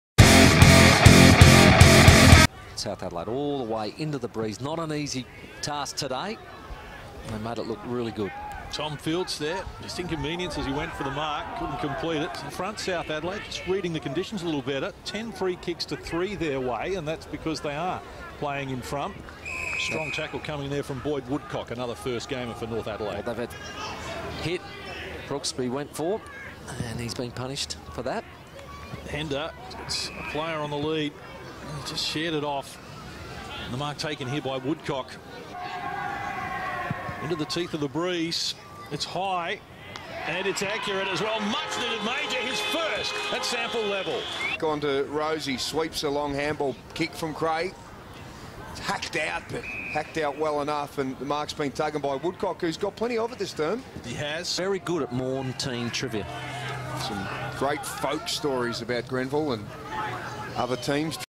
Round 15 BankSA Rookie - Boyd Woodcock (North)
From the Bute Paskeville Football Club, Woodcock looked lively in attack as he kicked a goal and collected 16 disposals and six tackles against South at Hickinbotham Oval.